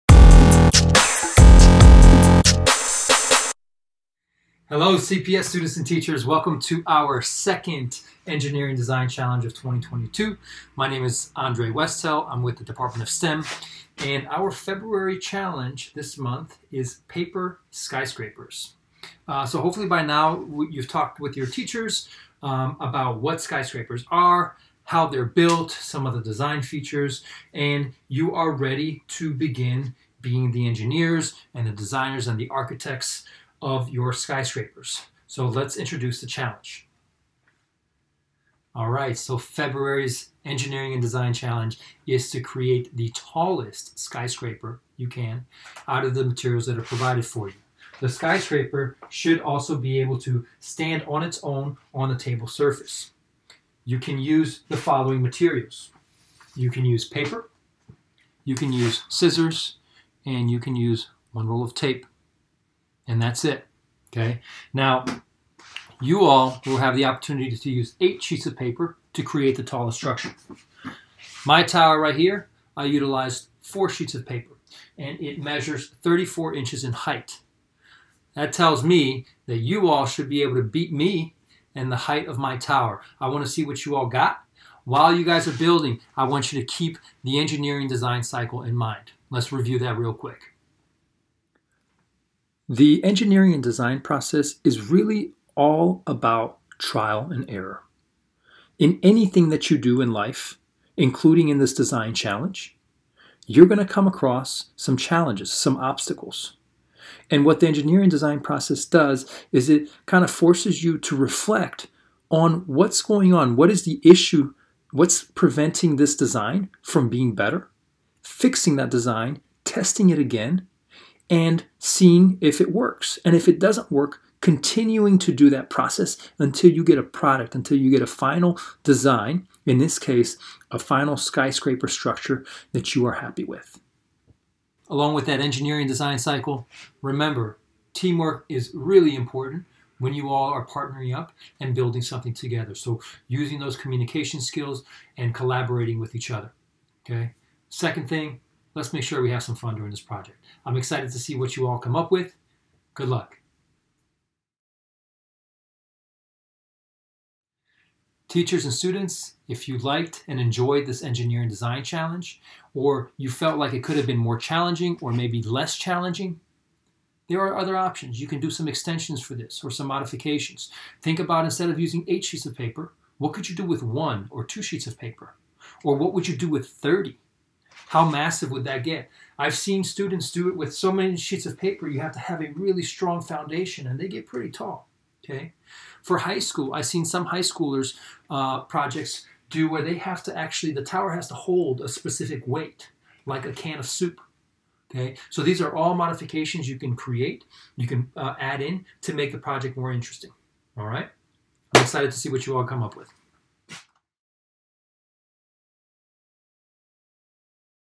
Paper Skyscrapers Engineering Design Challenge
Chicago Public Schools - Department of STEM - STE(A)M Schools Team
This is the second of a series of monthly instructional challenge videos that accompanies the  PDF document given to schools. This is a quick plug and play challenge meant to help schools build a culture of STEAM in their schools through the Engineering Design Cycle.